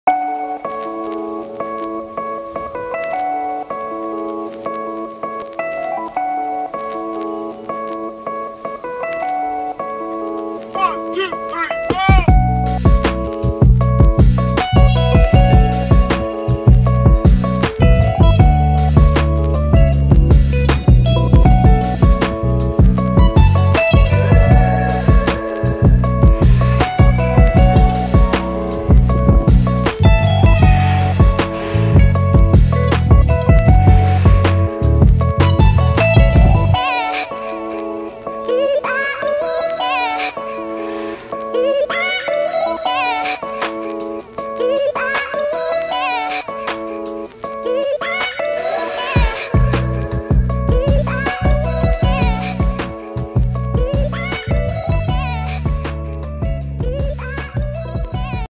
Short video Transformation from watercolor ink painting by Alex Solodov Art
Watercolor ink painting Transformation short video with music by @iksonmusic